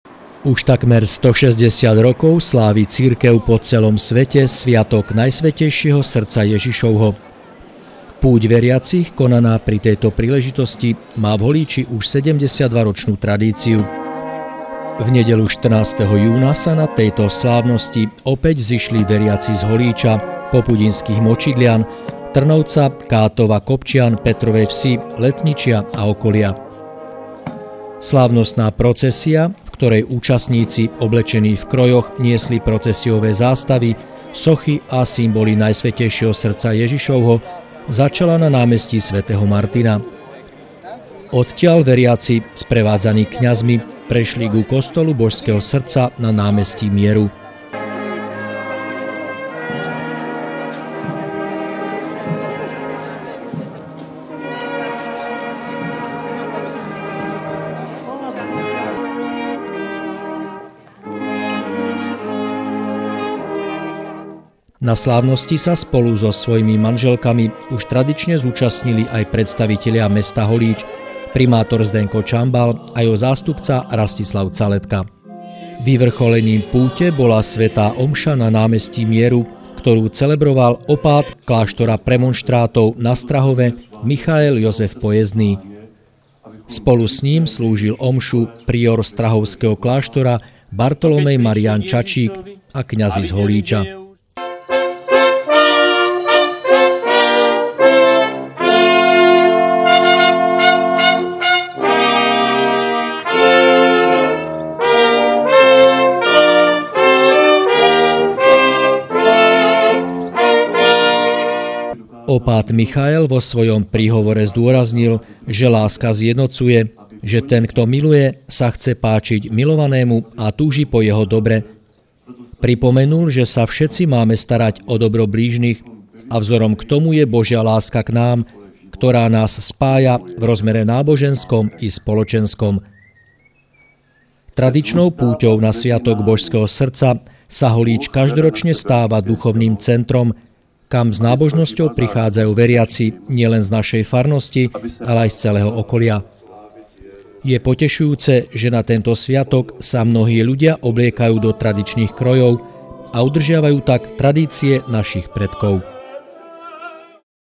Veriaci slávili sviatok Najsvätejšieho Srdca Jažišovho
1651